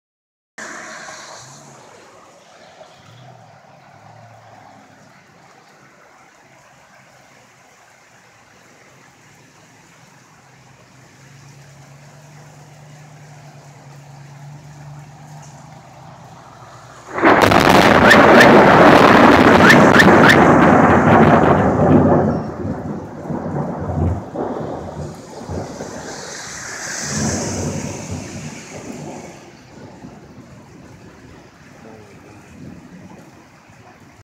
Thor was nearby.... I felt it! (6/28/15)
Just a video i shot out of my window today, and caught it at the right time... you notice the lighting right before the thunder clap. This video doesn't do it justice, it was very loud and intense... and shook the house. It scared the hell outta me... It followed by a power outage that lasted a few hours, and sirens being heard all over the place, not sure what happened though.